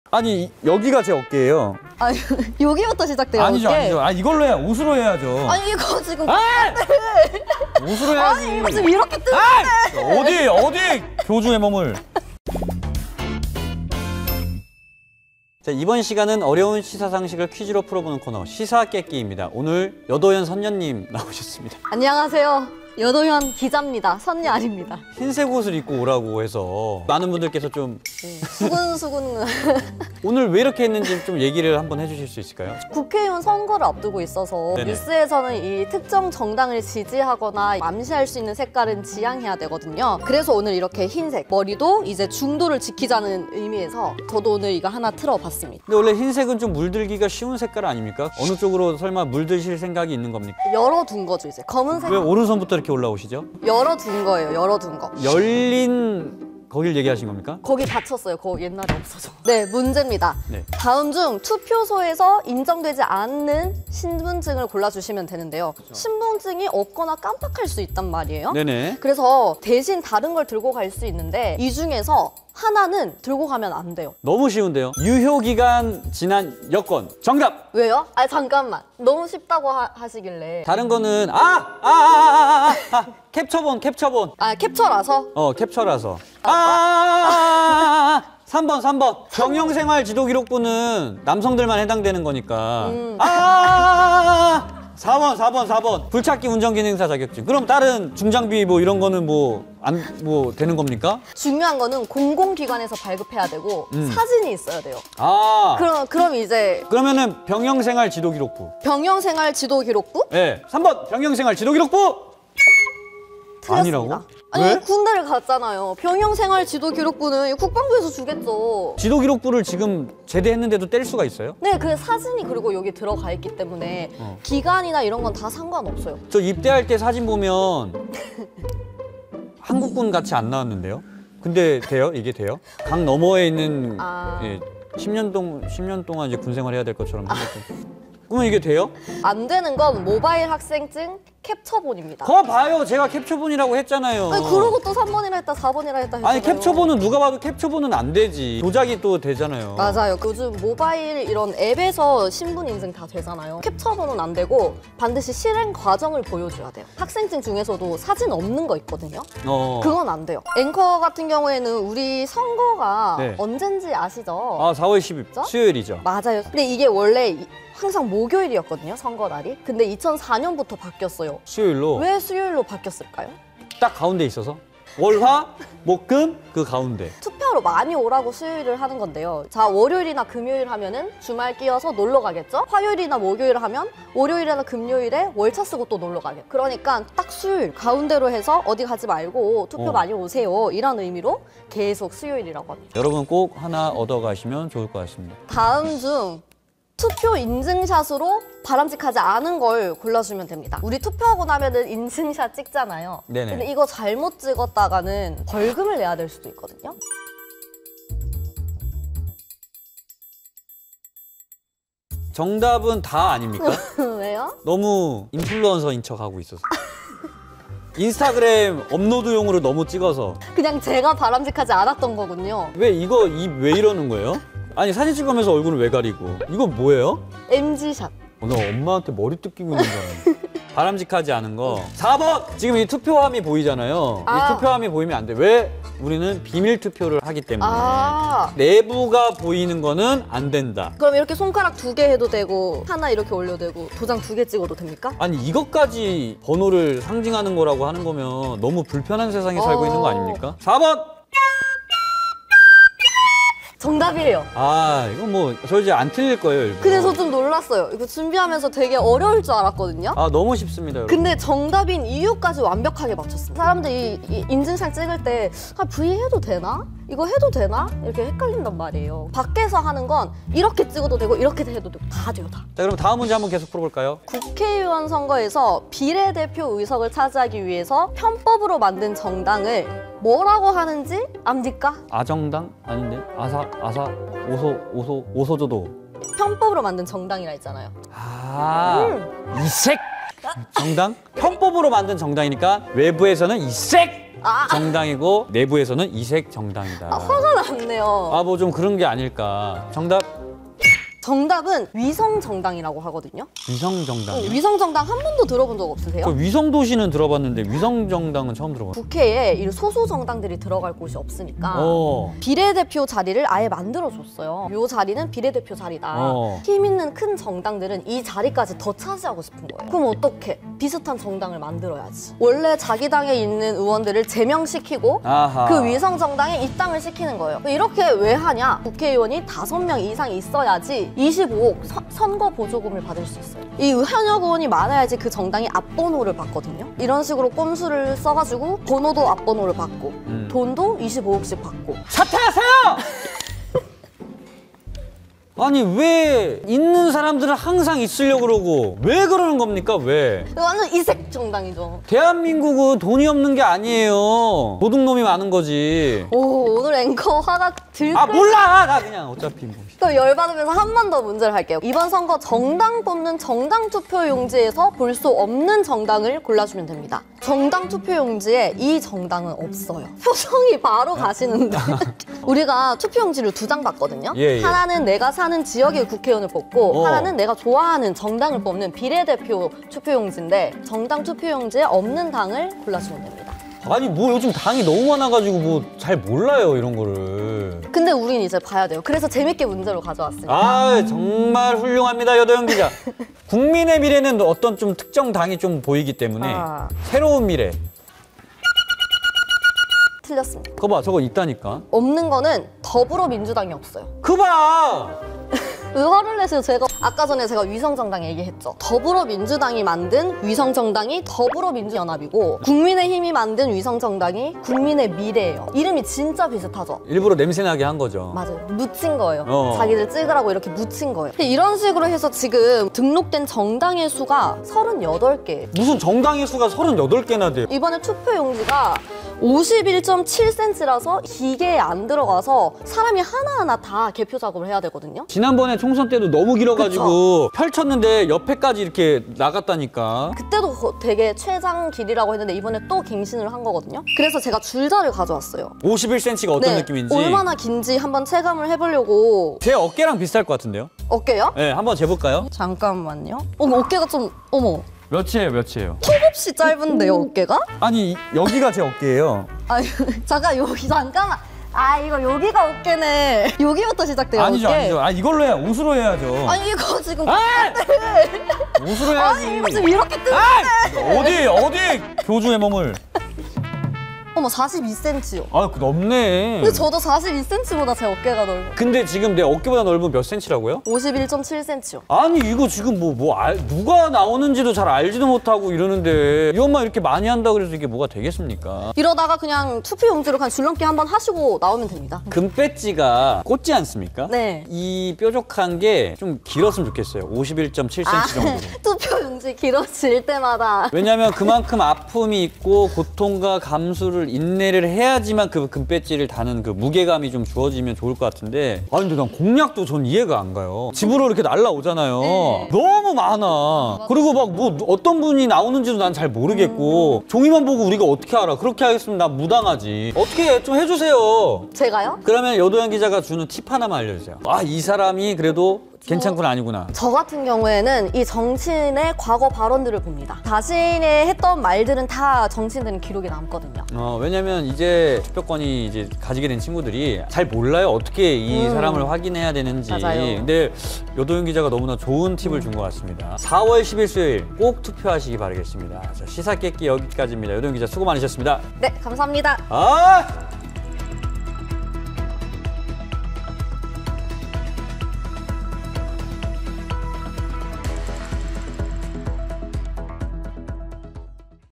투표는 너무 어!려!워! 🗳️남자 어깨 보다 긴 투표 용지가 등장했다? | 시사께끼
영상 내 잘못된 정보가 있어 수정 후 재업로드합니다.
사전에 확인하지 못한 점 사과드리며, 정확한 정보를 드릴 수 있도록 노력하겠습니다🙏